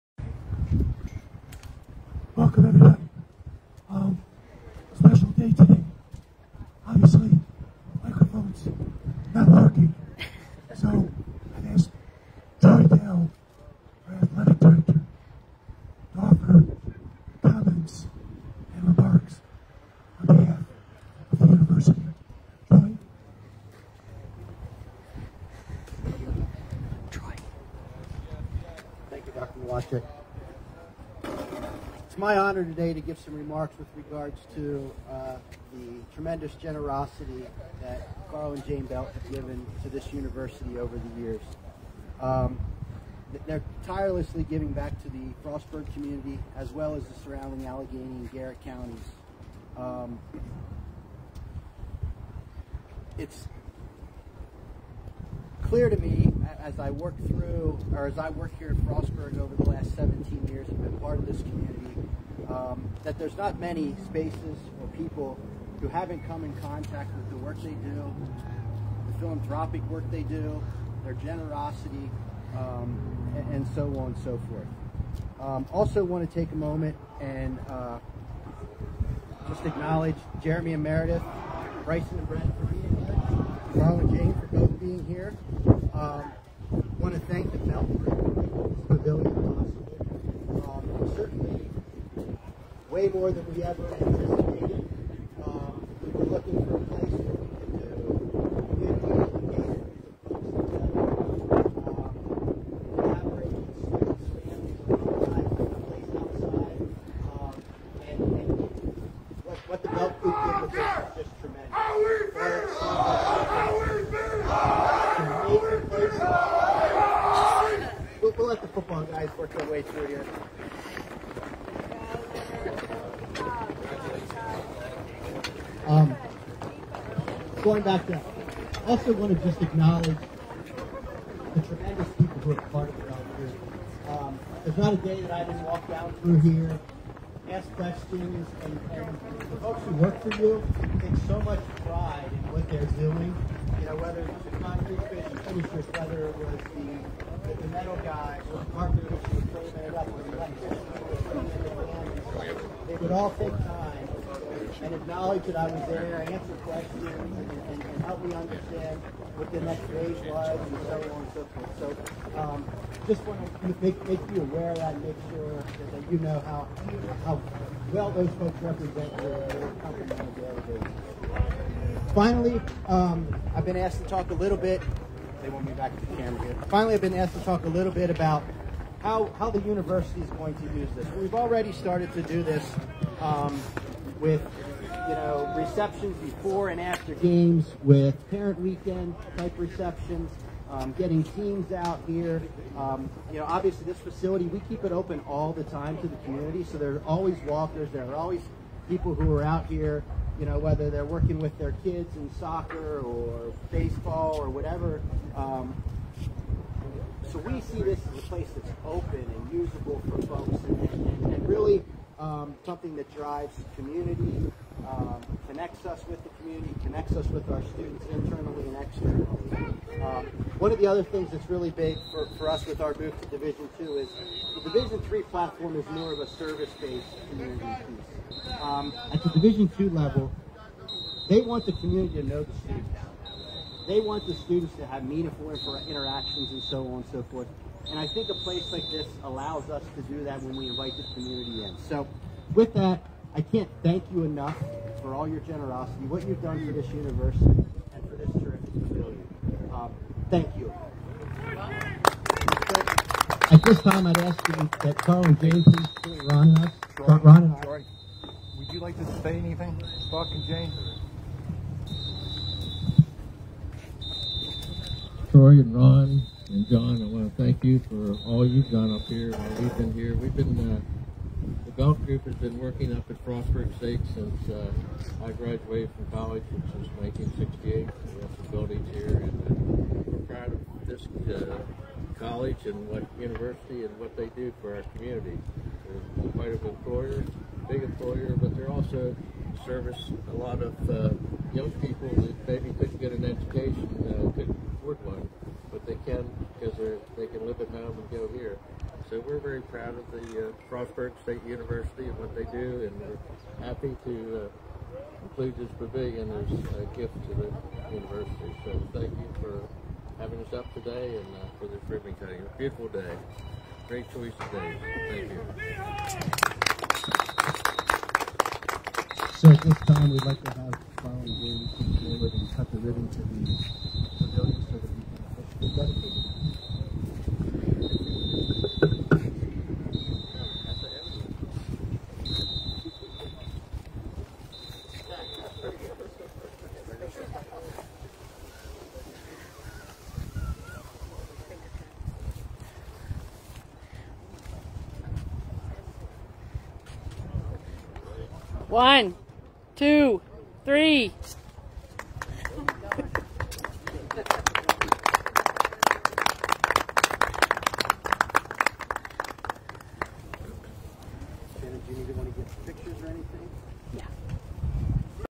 Bobcat Pavilion Dedication - Sunday, April 14, 2024
In honor of Carl & Jane Belt and their Belt Construction Group of Companies